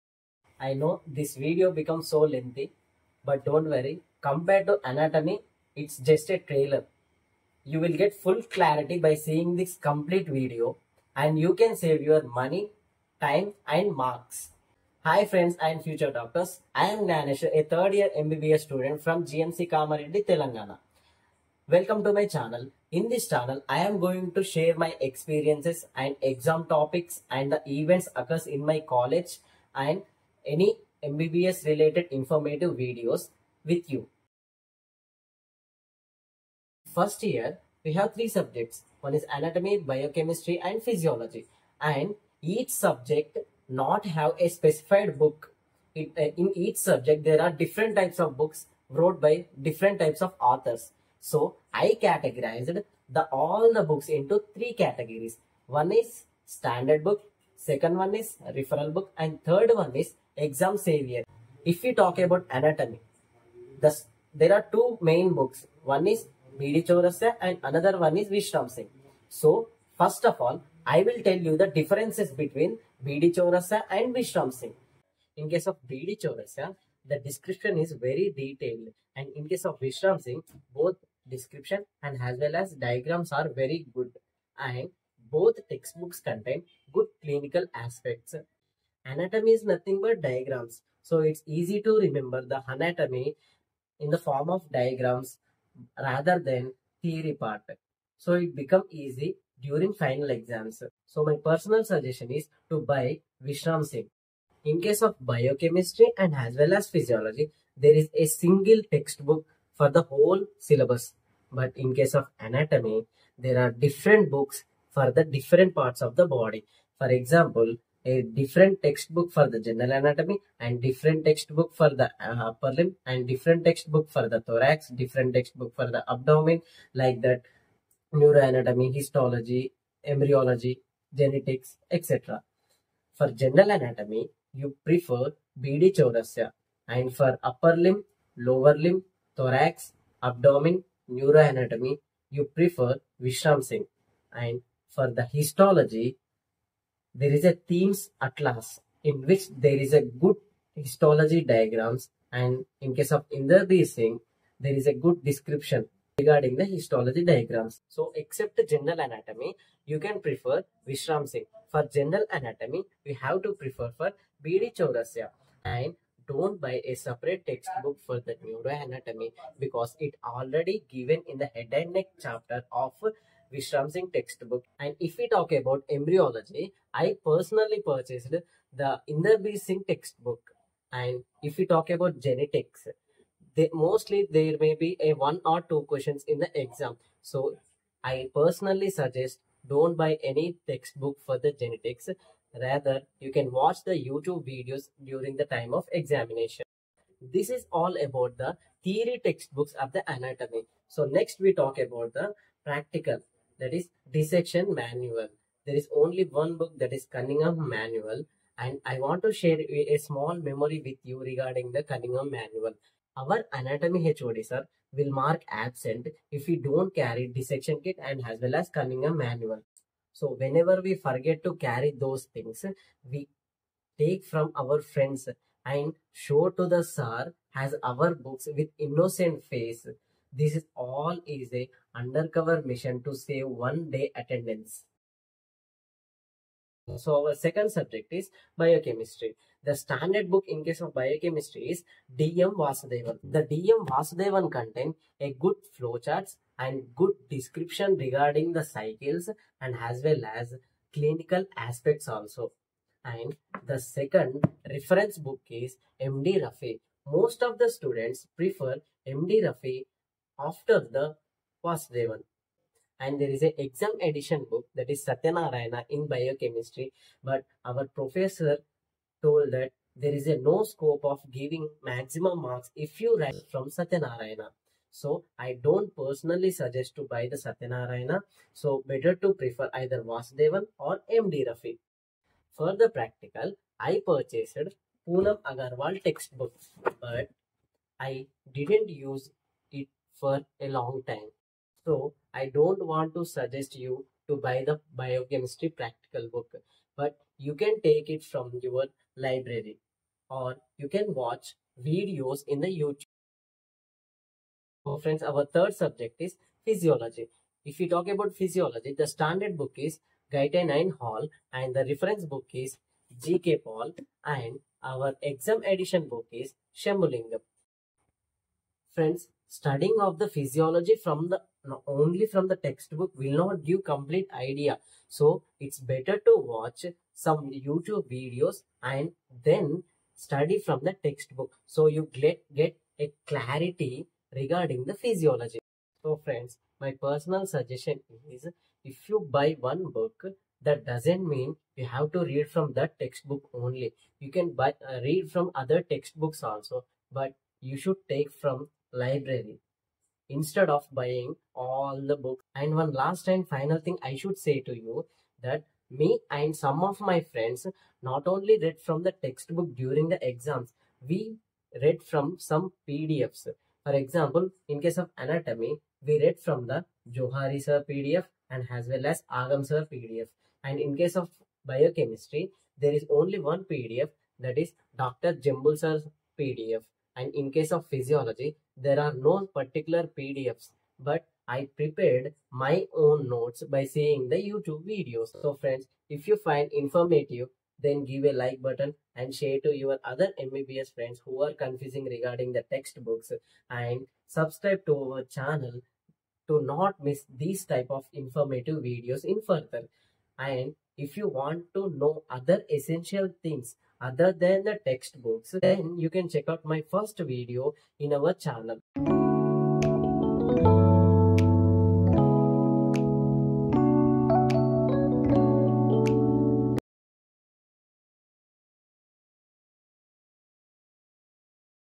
"Before You Buy 1st year MBBS Books,Watch This!"Don’t Waste Money 💵 |Choose the Right MBBS Textbook📒
MY SUGGESTION:⏩ ANATOMY: VISHRAM SINGH
                                ⏩  BIOCHEMISTRY: VASUDEVAN
                                ⏩   PHYSIOLOGY:GK PAL
📌ANATOMY:
    - General Anatomy (BD Chourasia from library)
    - Upper Limb
    - Lower Limb
    - Thorax
    - Abdomen & Pelvis
    - Neuroanatomy
- All these topics are covered by Vishram Singh.
- Additional topics and textbooks mentioned:
    - Histology - Thieme Atlas/Inderbir Singh
    - Embryology - Inderbir Singh
    - Genetics - no need of textbook

📌BIOCHEMISTRY :

STANDARD          REFERENCE        EXAM EDITION
DM VASUDEVAN    MD RAFI        SATHYA NARAYANA

PRACTICE: YOUTUBE VIDEOS OR POONAM AGARWAL

📌 PHYSIOLOGY:

    - GUYTON & HALL (STANDARD)
    - GK PAL (REFERENCE)
    - SEMBULINGAM (EXAM EDITION)
- Practice book: GK PAL